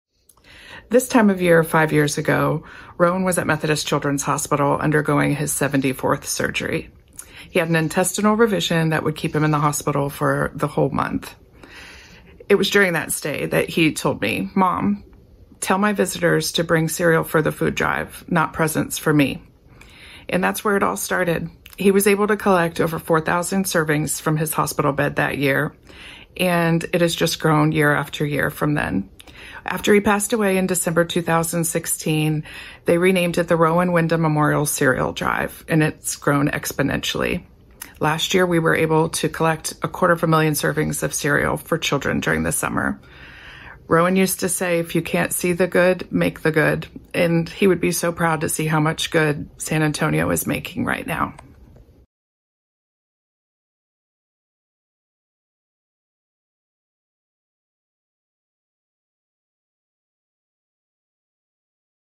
How Rowan Windham Came to Love the Cereal Drive
Carrie Windham shares the story of how her son Rowan's passion for the cereal drive began.